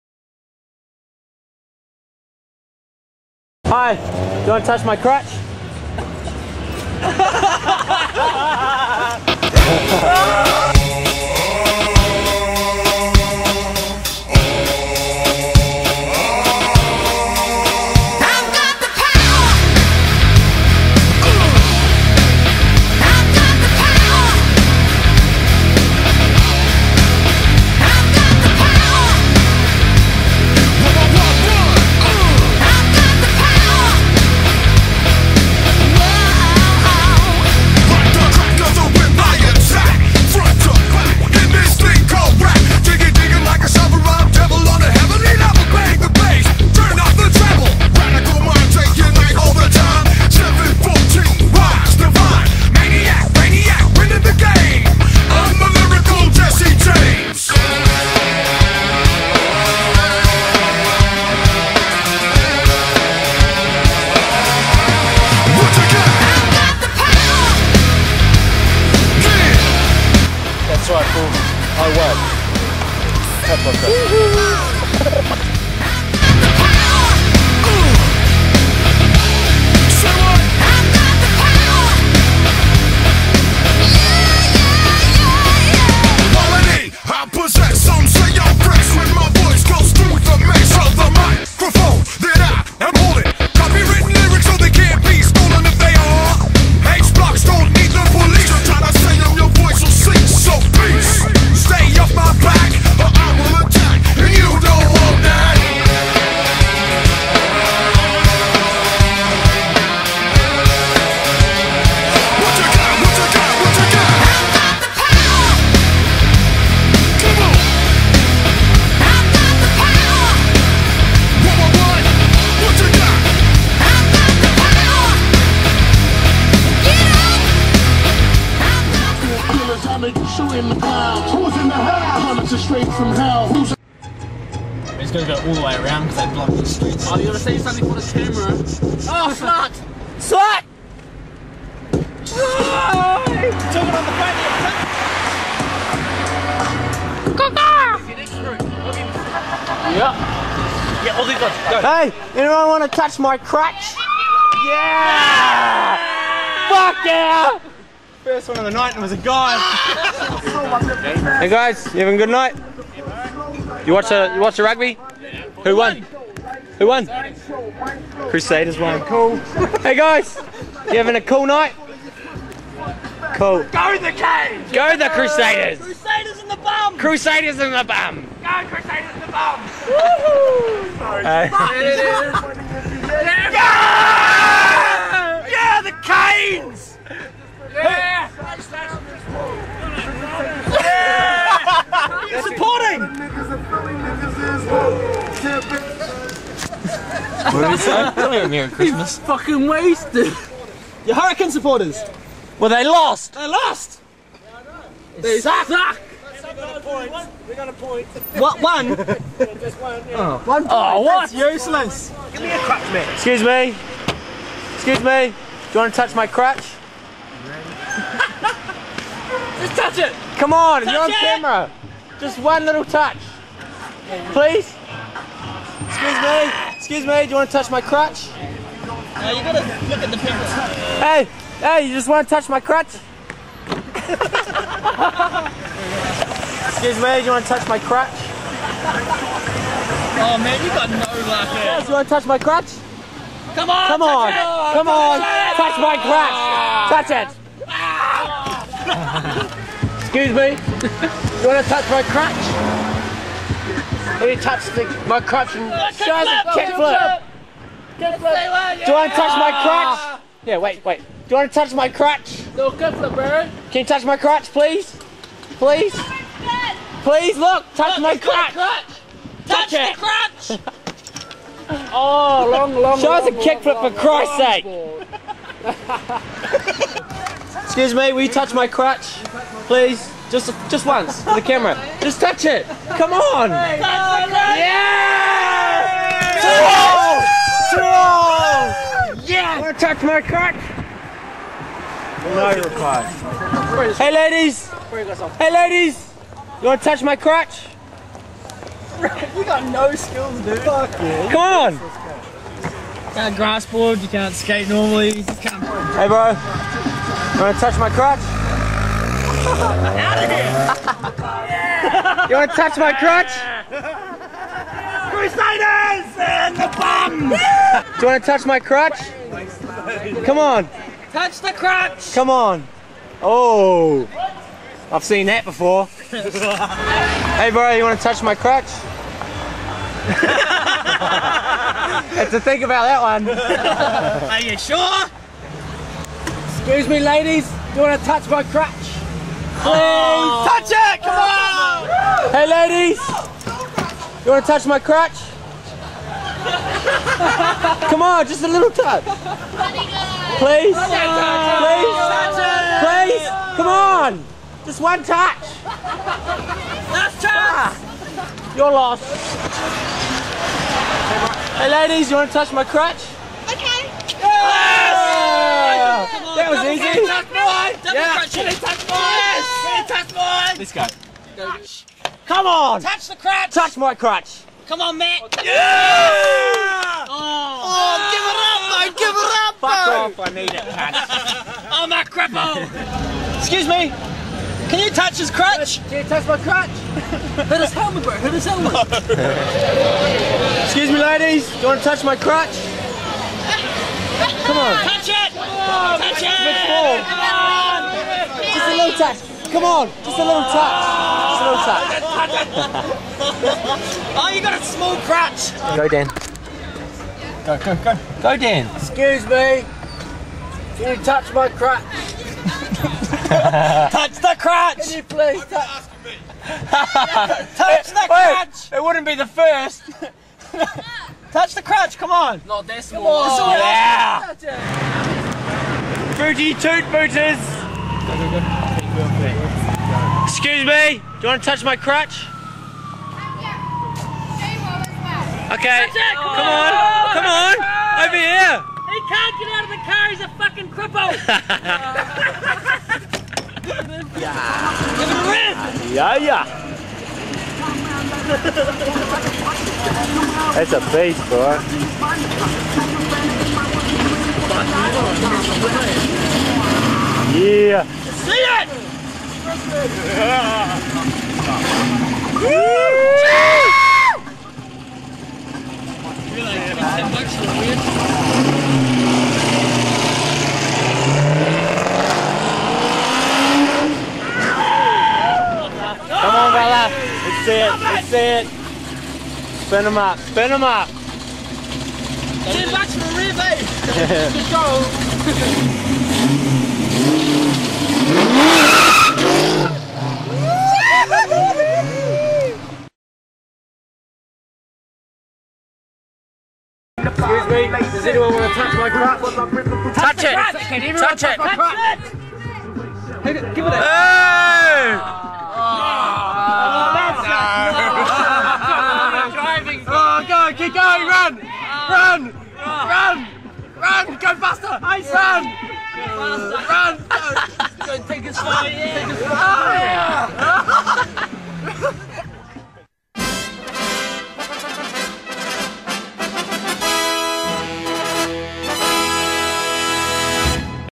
N.S.C Prank... Touch My Crutch.
cruzing the streets of wellington tryign to get people to touch the crutch and other antic's